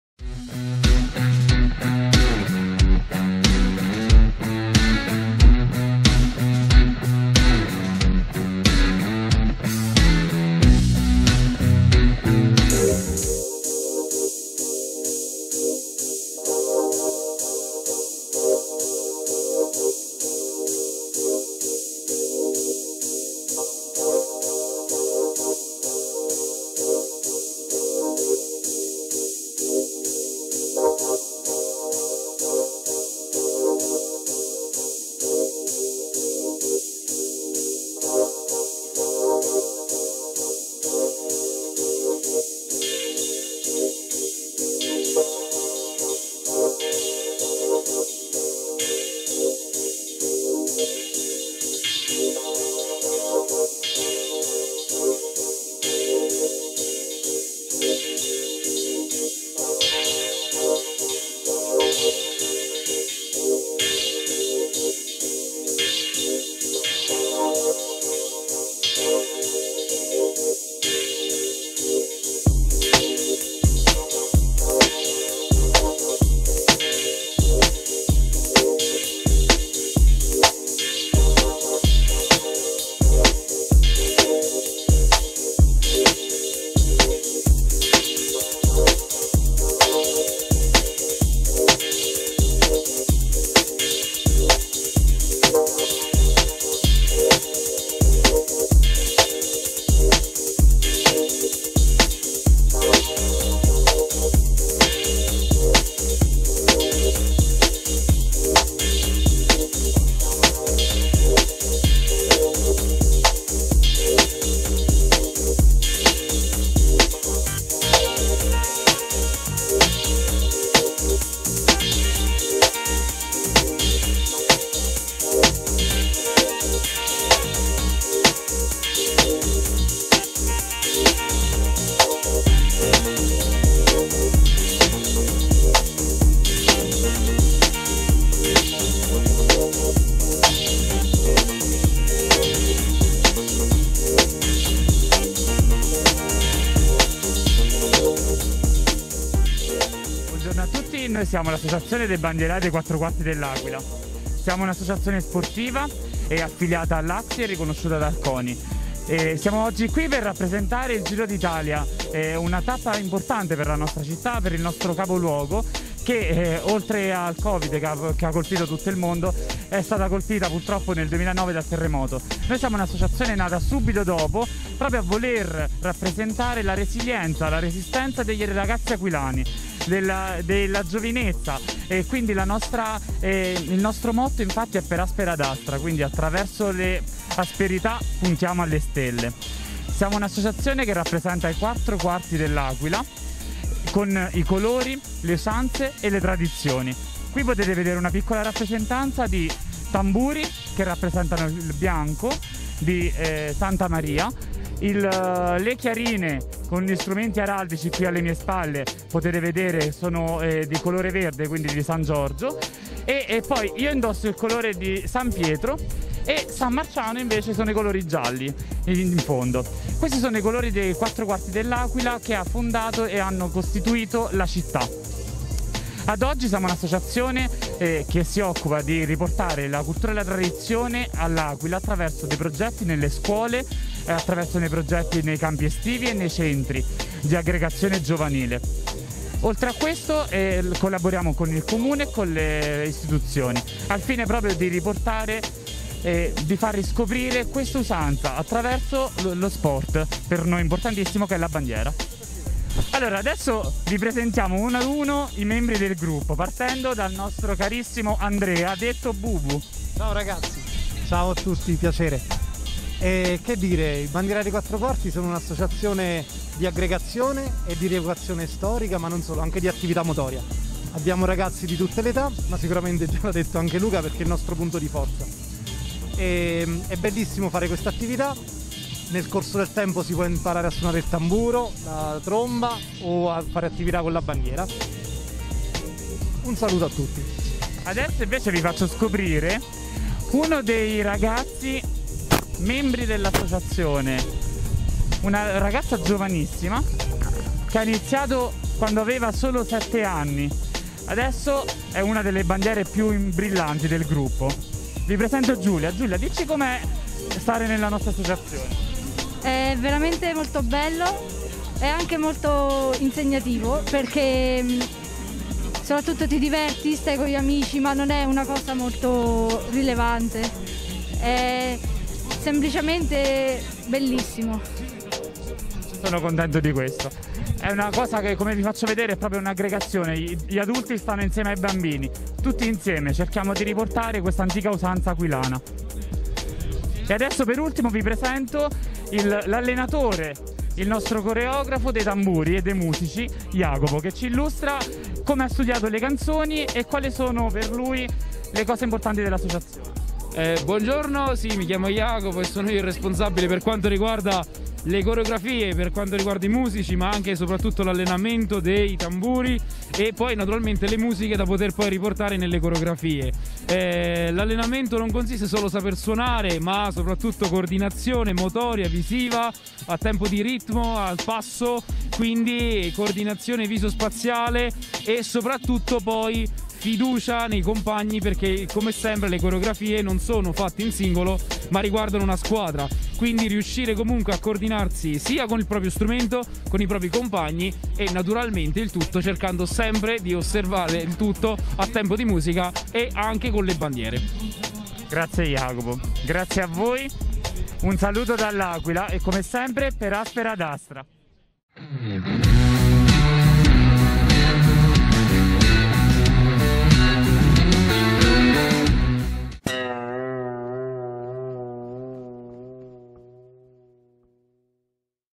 Giro d'Italia 2021 - ASD Bandierai dei quattro Quarti - Tappa 10 (L'Aquila - Foligno)
Giro d'Italia 2021 - ASD Bandierai dei quattro Quarti - Tappa 10 (L'Aquila - Foligno) - 17 maggio 2021 -  A cura di Antonio Abbruzzese,  Giuseppe Girolamo, Filippo Mazzullo, Riccardo Cristiano, Gianni Colacione.